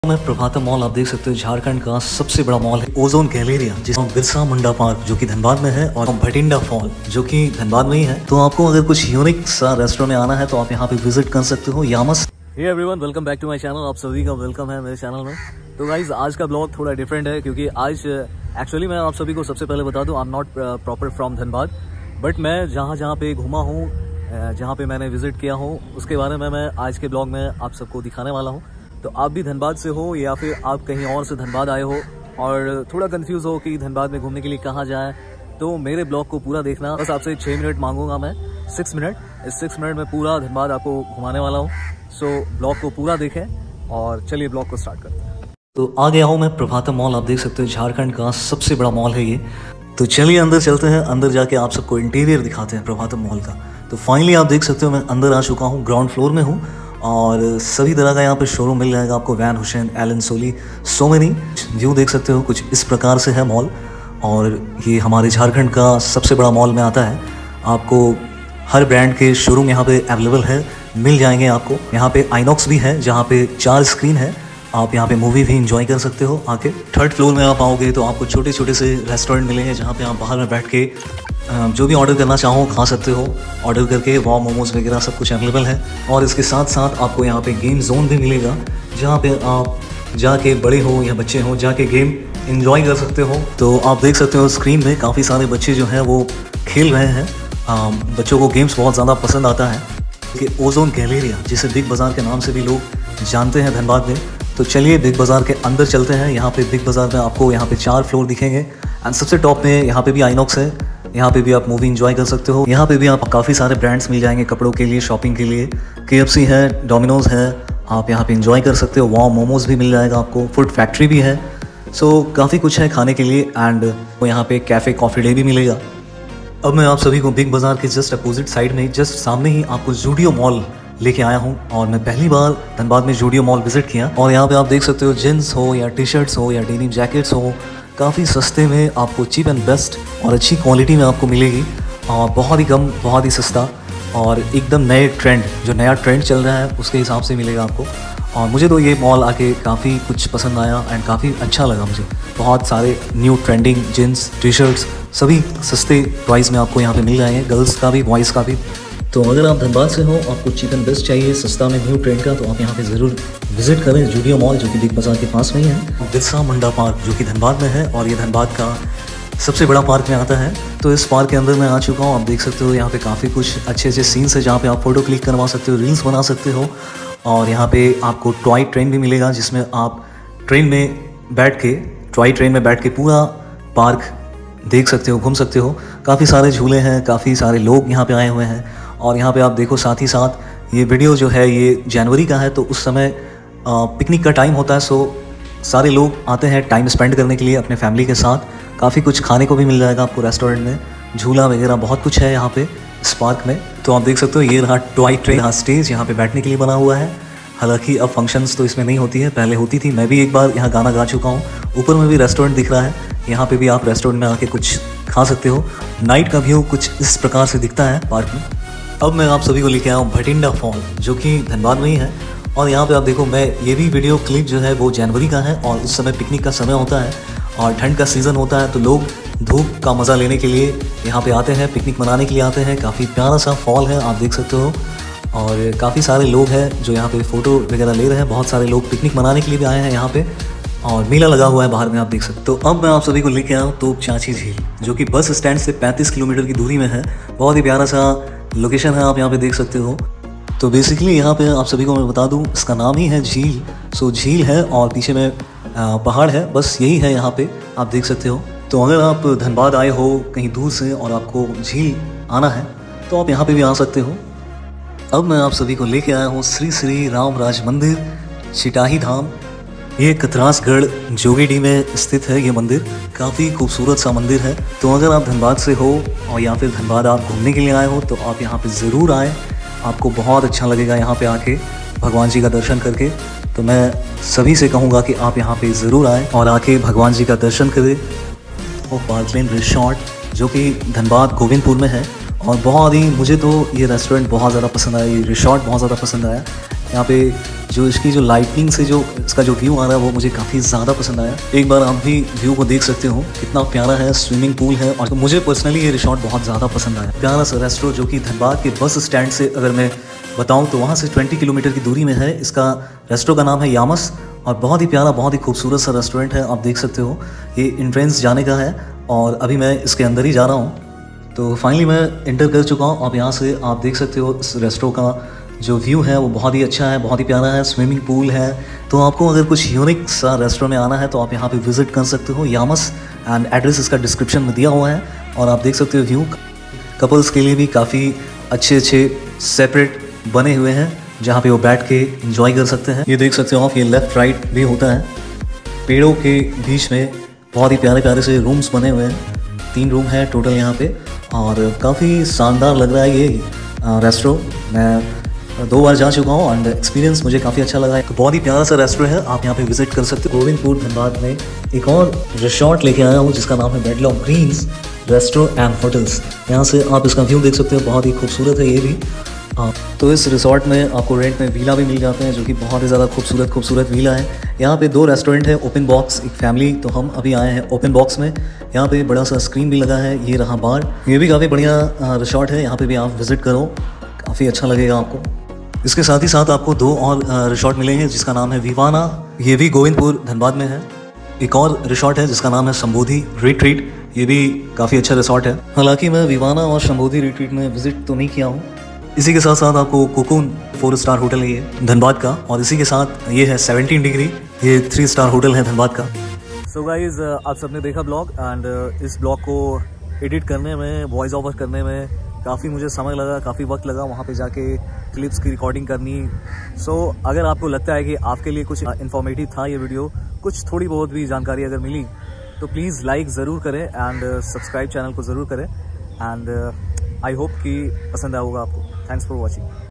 Top 10 Famous place in Dhanbad || Dhanbad tourist place
Prabhatam mall
Ozone galaria
zudio mall
Birsa munda park
Bhatinda Fall
Topchanchi jhill
Shree Shree Ram Raj Mandir
parklaneresort
yamas
wedlock greens restro and hotels
vivana
sambhodhi retreat
Cocoon hotel
17 degree hotel